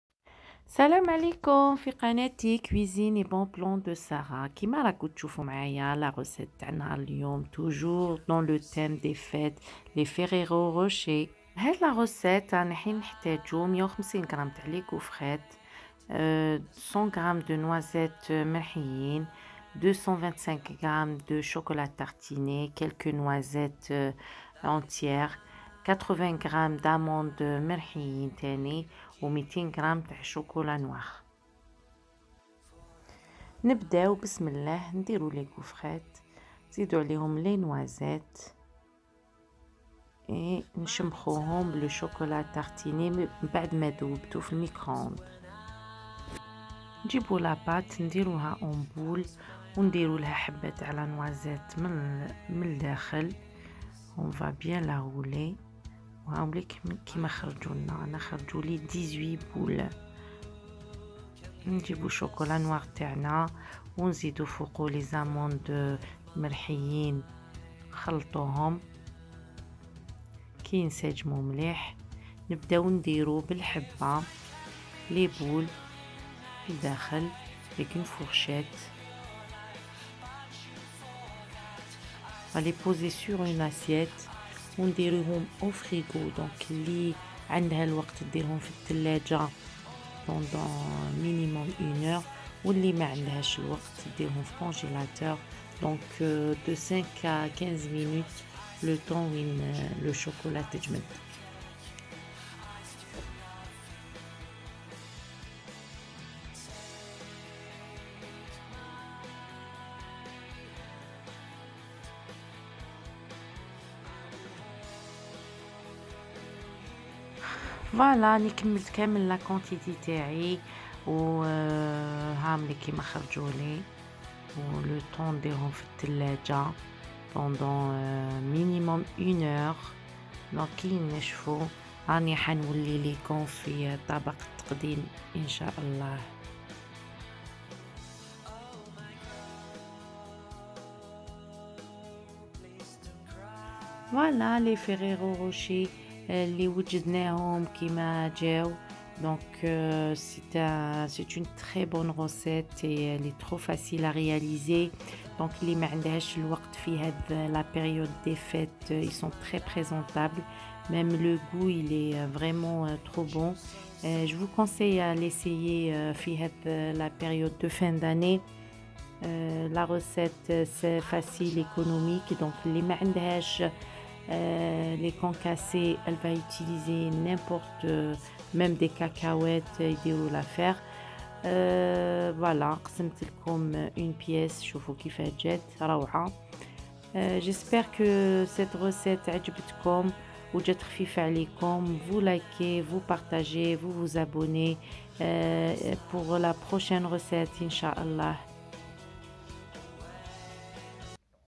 FERRERO ROCHER, GÂTEAU SANS CUISSON TROOOOP BON idéal pour les fêtes
salam 3likoum j'espère que vous allez bien inchalah.
 enfin une recette top pour des ferrero rocher, gâteaux sans cuisson
simple, rapide tooooooooooop économique.

ingrédients :pour la pâte,
150 g de gaufrette.
100 g de noisette mixée.
225 g de chocolat à tartiner.
20 à 30 ml de lait.

pour la farce:
quelques noisette entières.

pour la décoration :
200 g de chocolat noir.
80 g d'amandes mixées grossièrement.